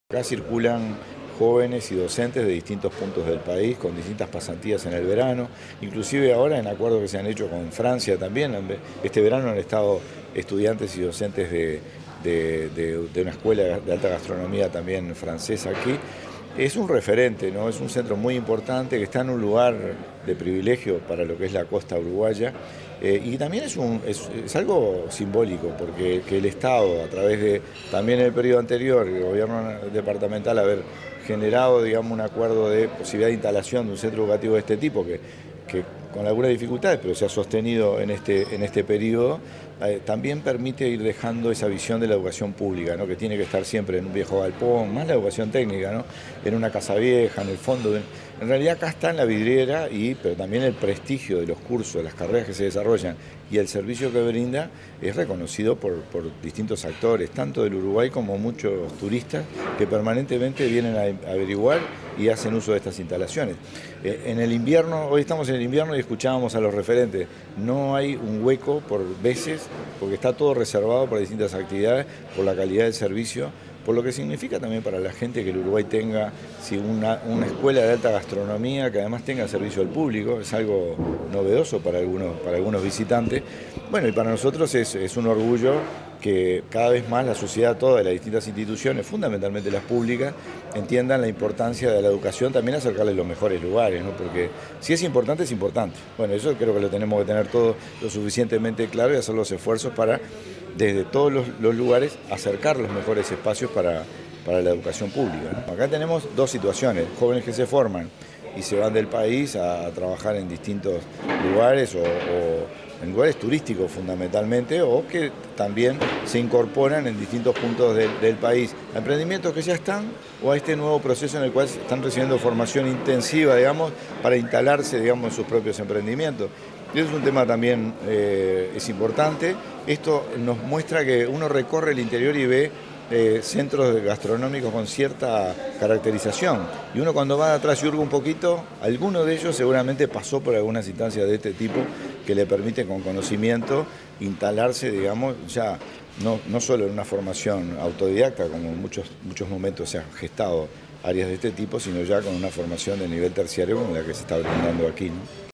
Escuela gastronómica de UTU en el este del país forma a 200 estudiantes
La Escuela de Alta Gastronomía de UTU en Punta del Este es la única de práctica del país que conjuga formación profesional con experiencia laboral para 200 estudiantes. Funciona en invierno y verano con servicio de restorán y eventos privados. El titular de ANEP, Wilson Netto dijo que el centro cuenta con un excelente servicio y agenda a tope todo el año. Entiende fundamental que haya una propuesta así en la educación pública.